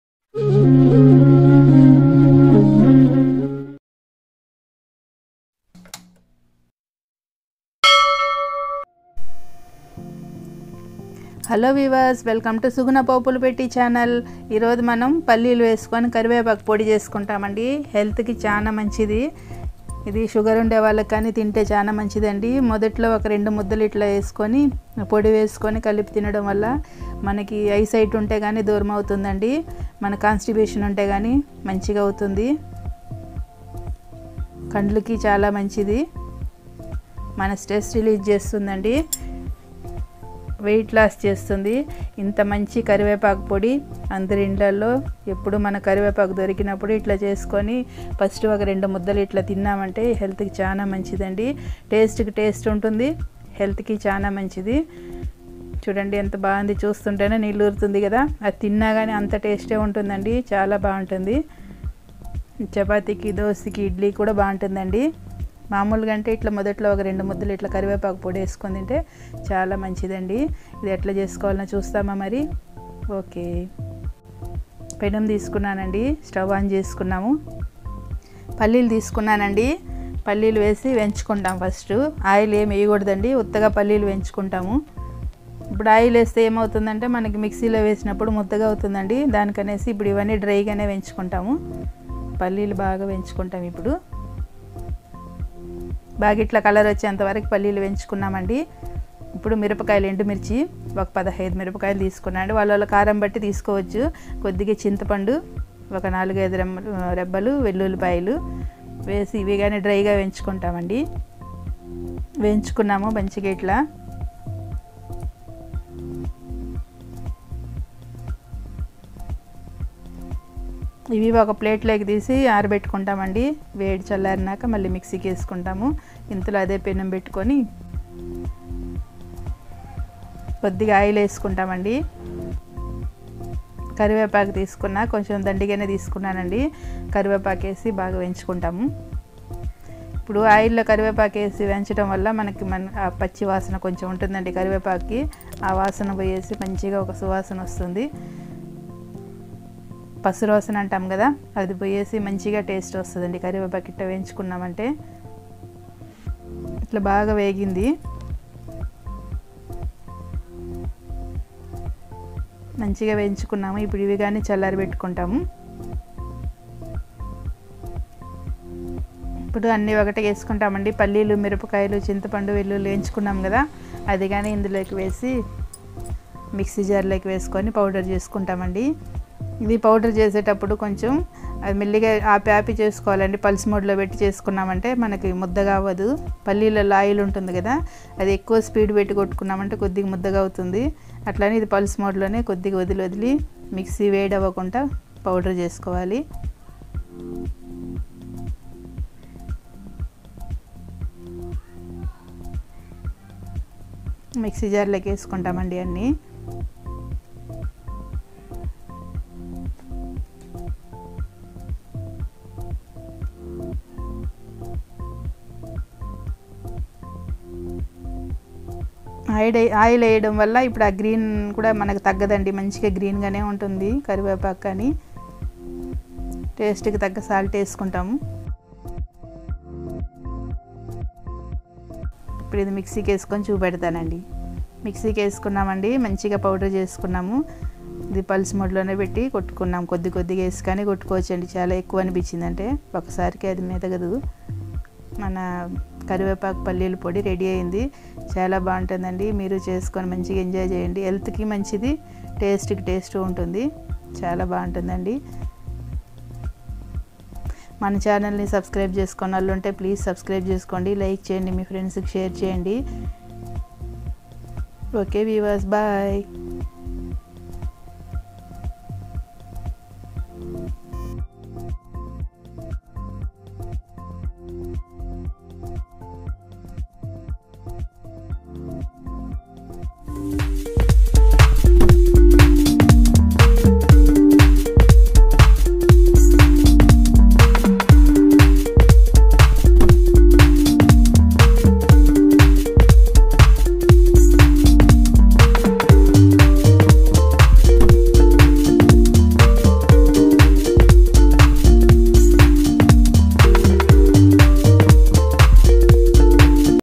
Karivepaku Palli Podi | కరివేపాకు మరియు పల్లీల తో పొడి | Curry Leaves and Groundnut Recipe 😋
In this Video i have shown how to Prepare Karivepaku Palli Podi | కరివేపాకు మరియు పల్లీల తో పొడి | Curry Leaves and Groundnut Recipe 😋